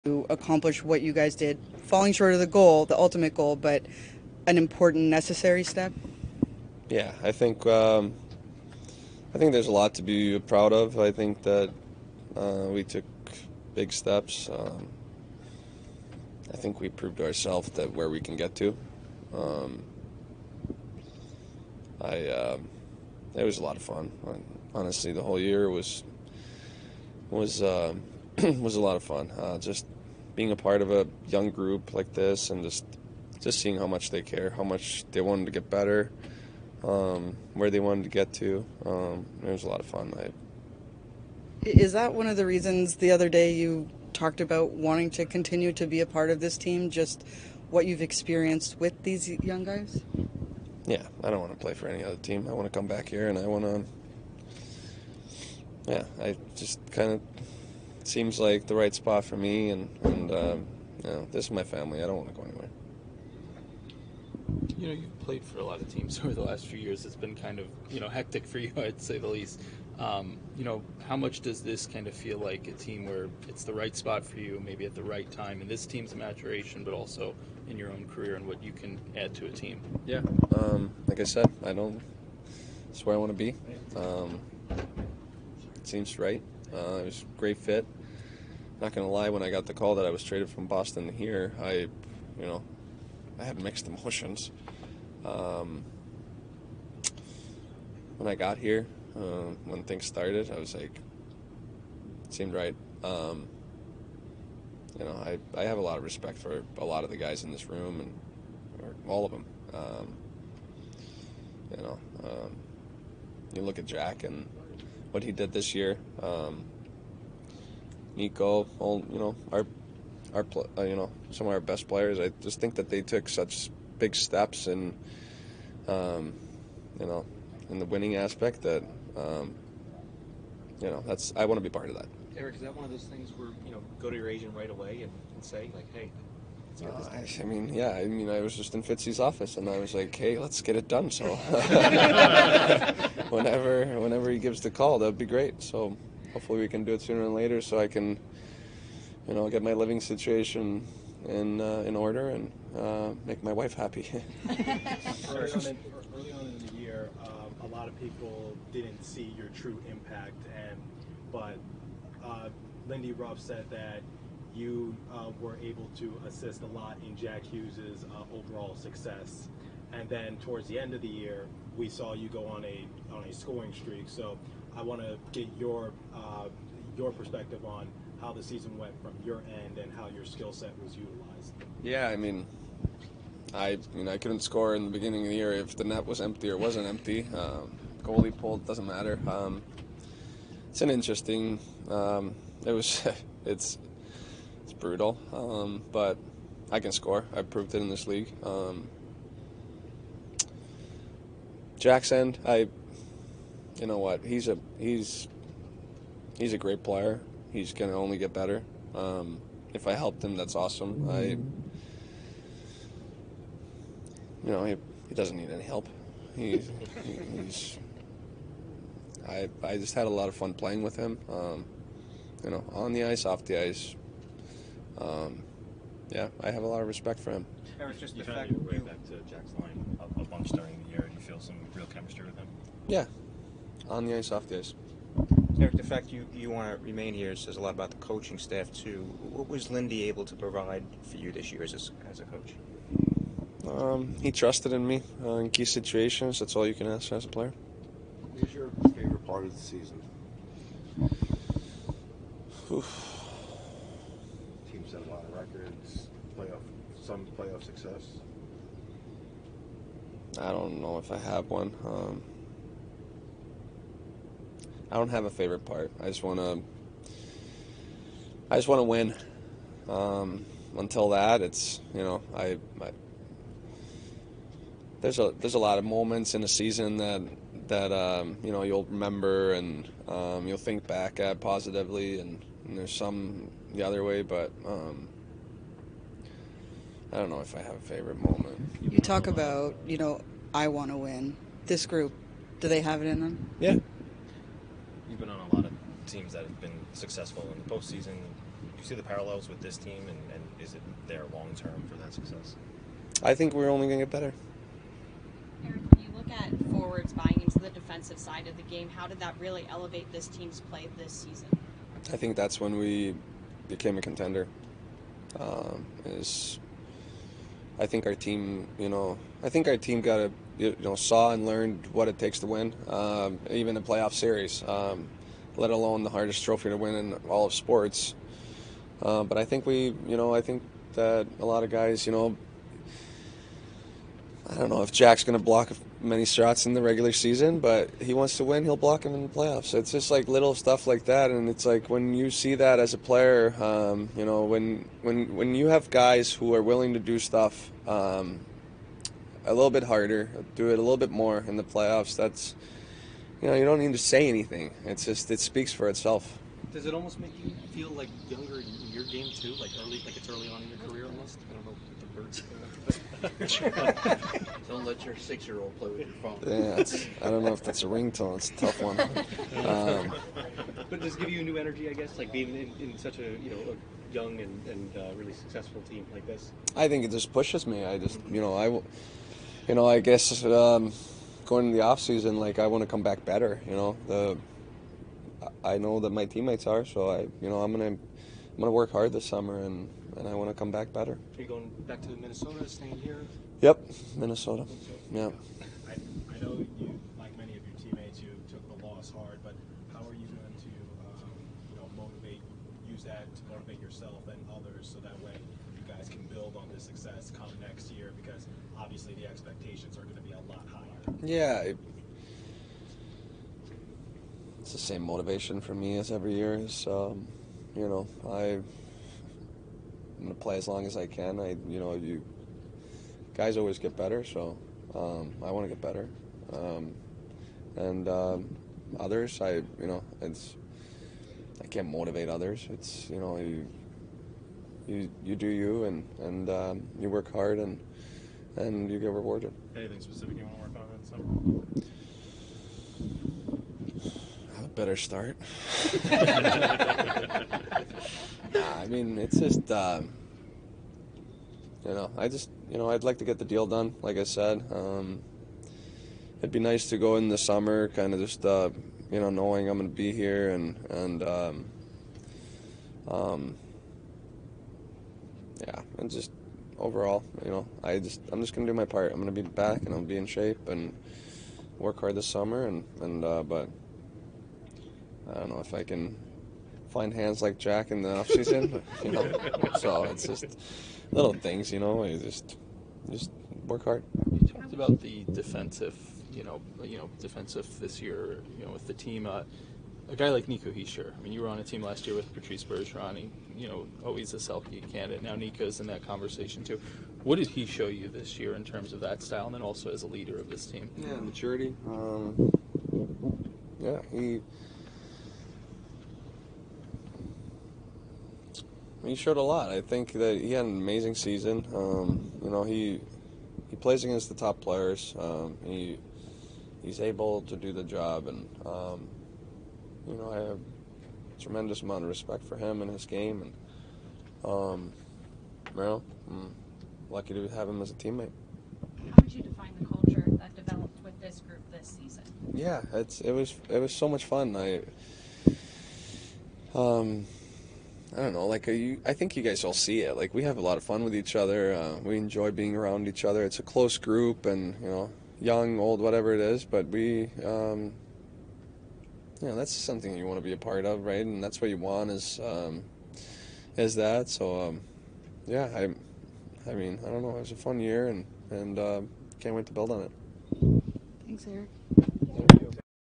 Erik Haula Exit Interview | NEW JERSEY DEVILS
Erik Haula speaks to the media at the end of the season.